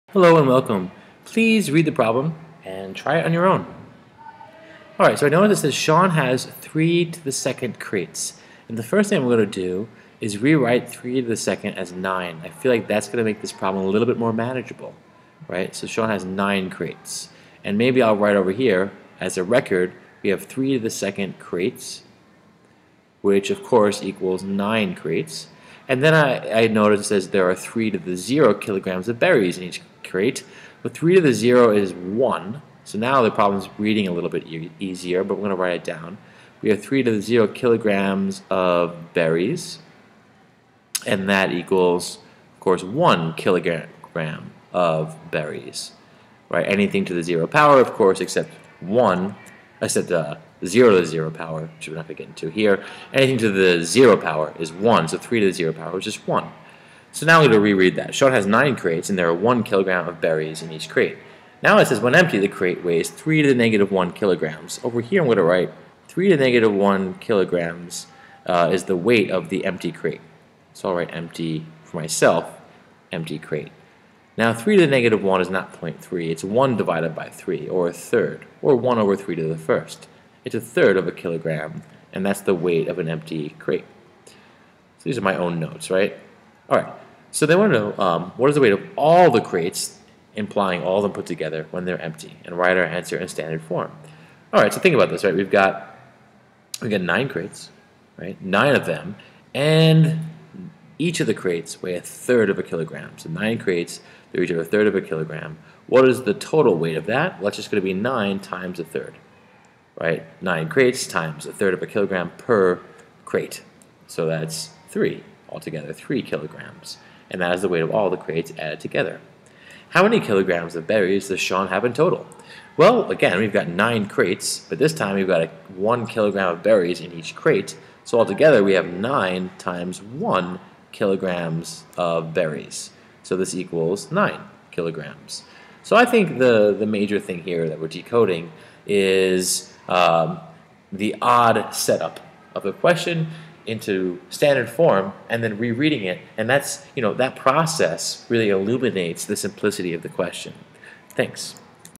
Exponent Review 01 and 02
In this video, we analyze a problem involving exponents